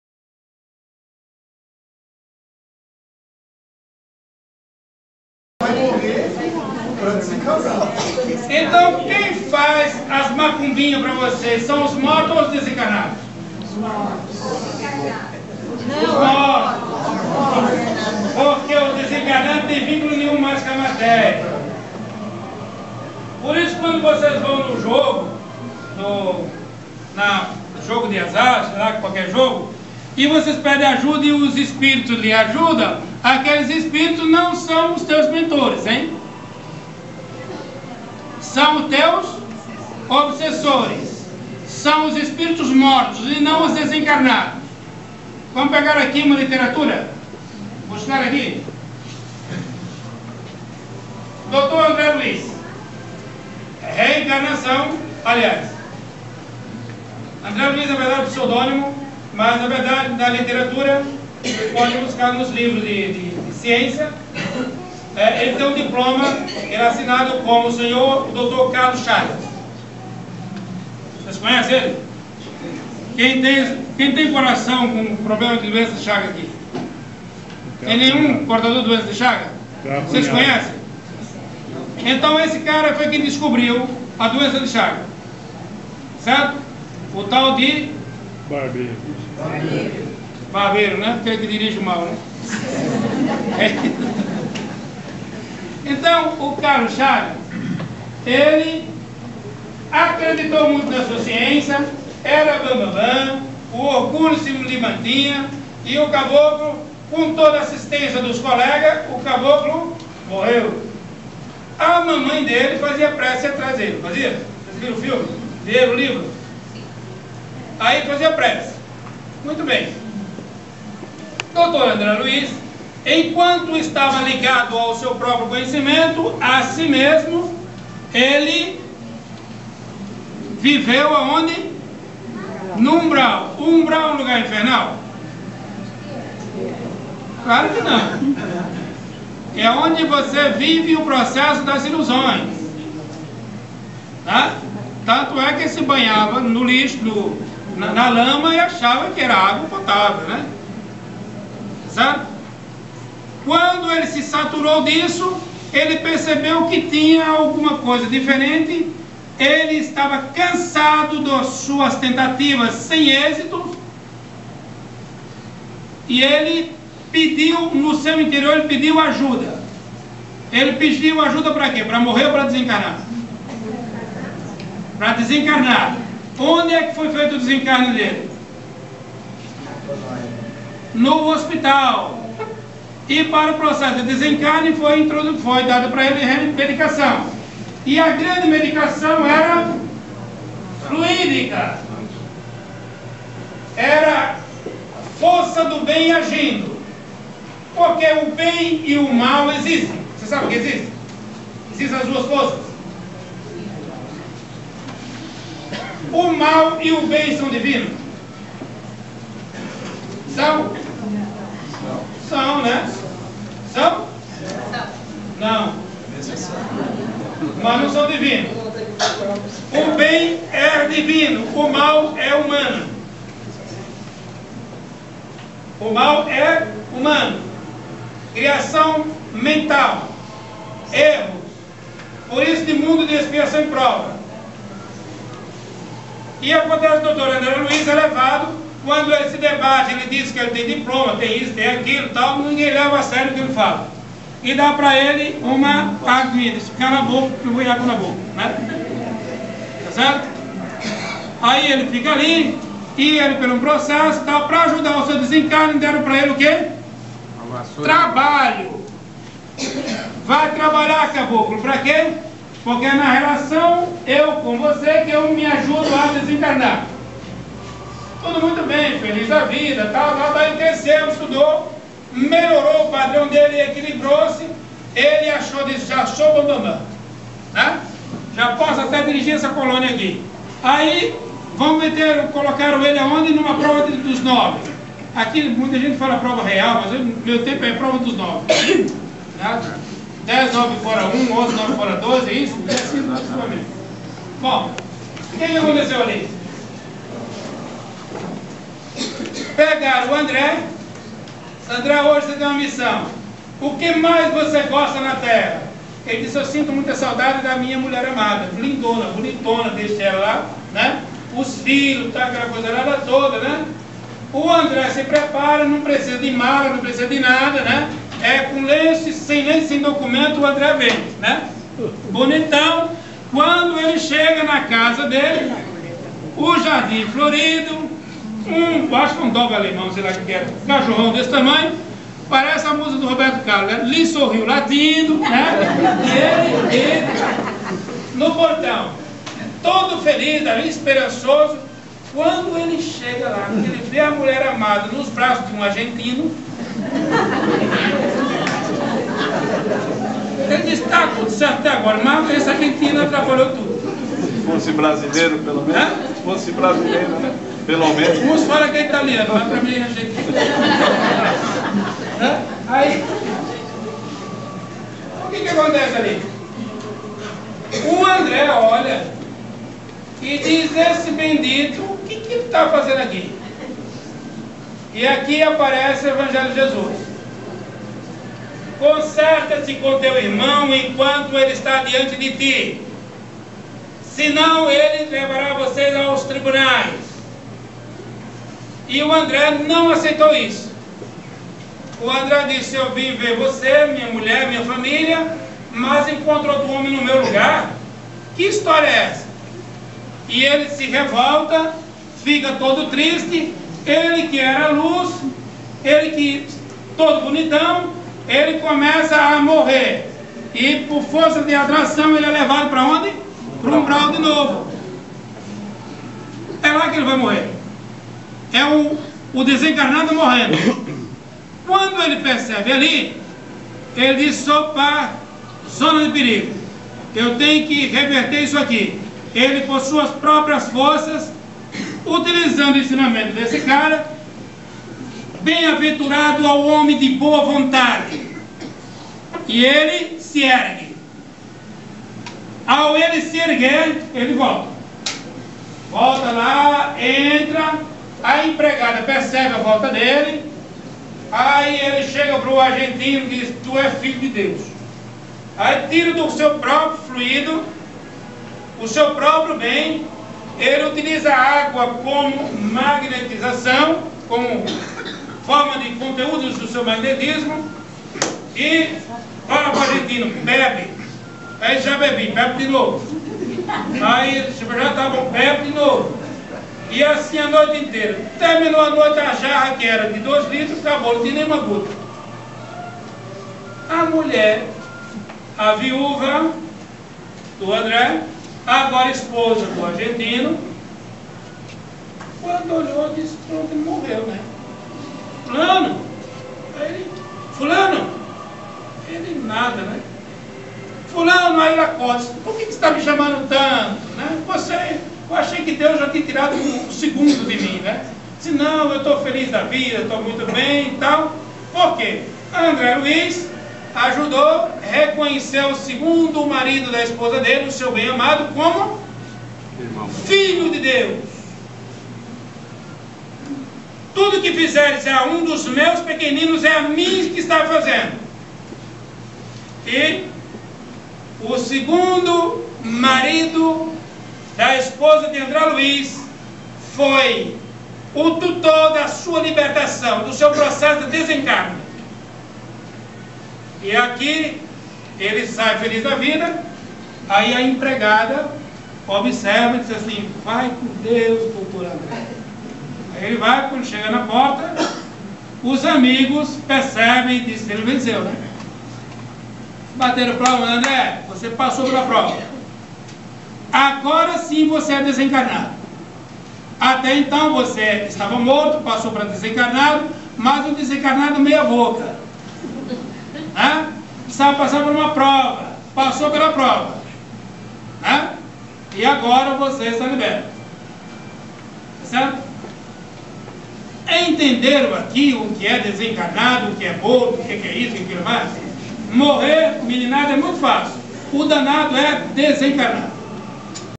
Palestras IECIM - Série Personalidades: André Luis
Palestras e atendimentos realizados no Instituto Espírita Cidadão do Mundo por Laerson Cândido de Oliveira.

Este vídeo faz parte de uma série de narrativas sobre as encarnações de alguns espíritos famosos, que despertam a curiosidade de todos. A história de André Luis ensina-nos muito sobre a imperfeição humana e como evoluir em nossas relações com o próximo.